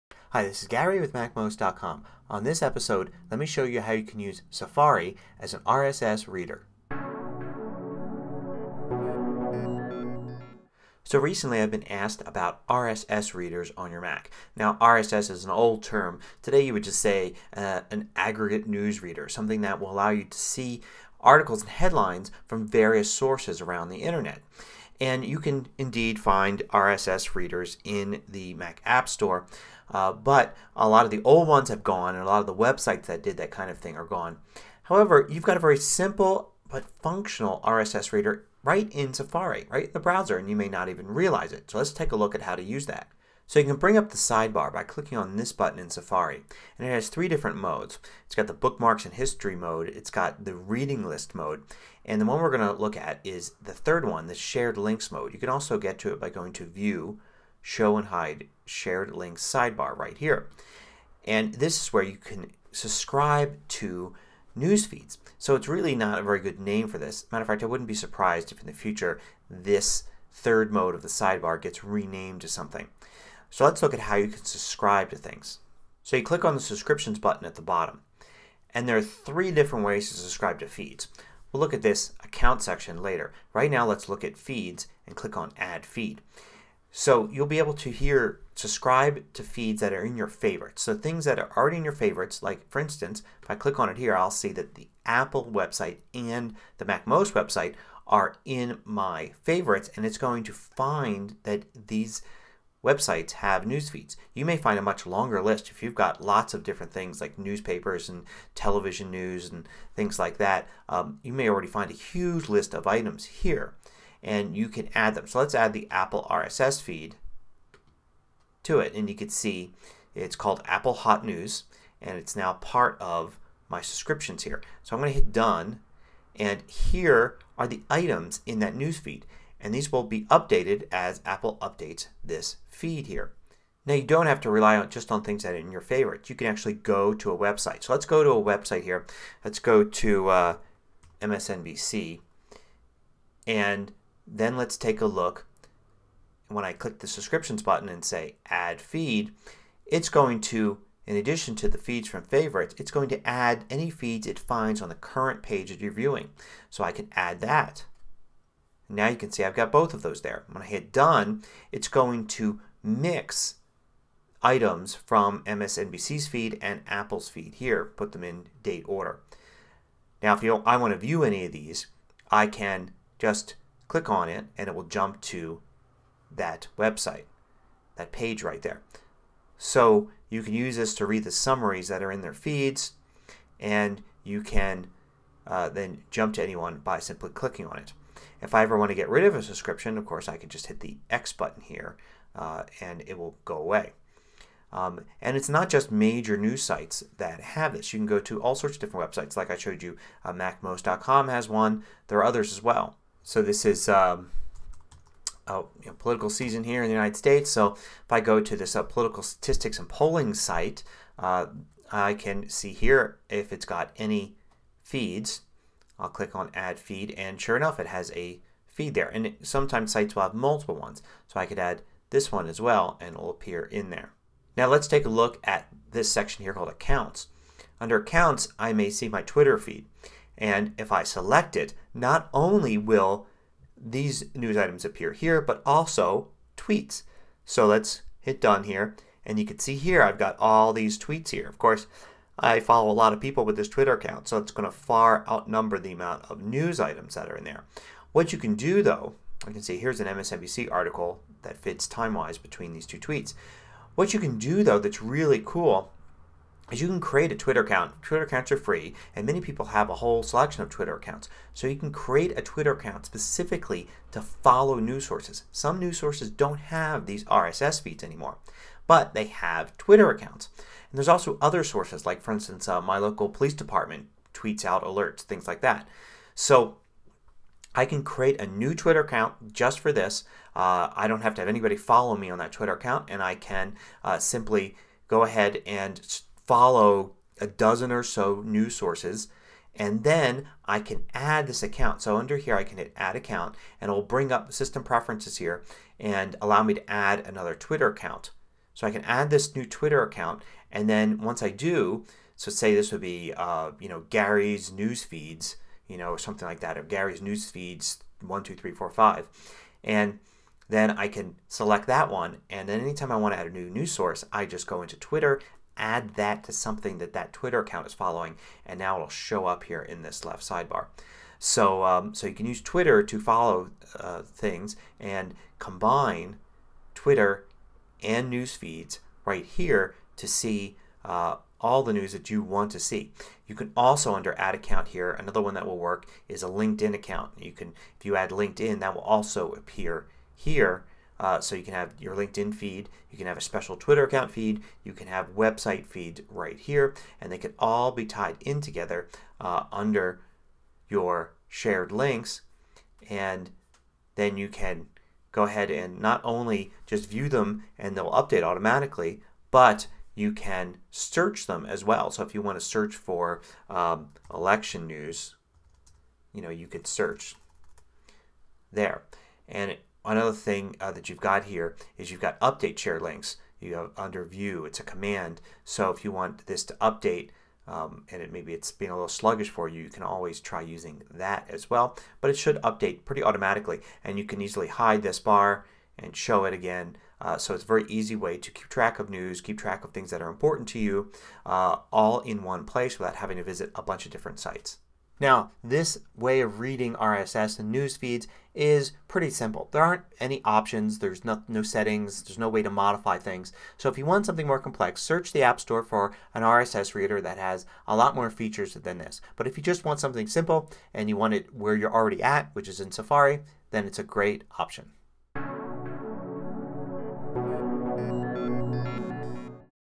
Using Safari As an RSS News Reader (#1200)
You can also include Twitter and LinkedIn feeds. This allows you to see the news from your favorite sources all in one place without needing to go to individual web sites.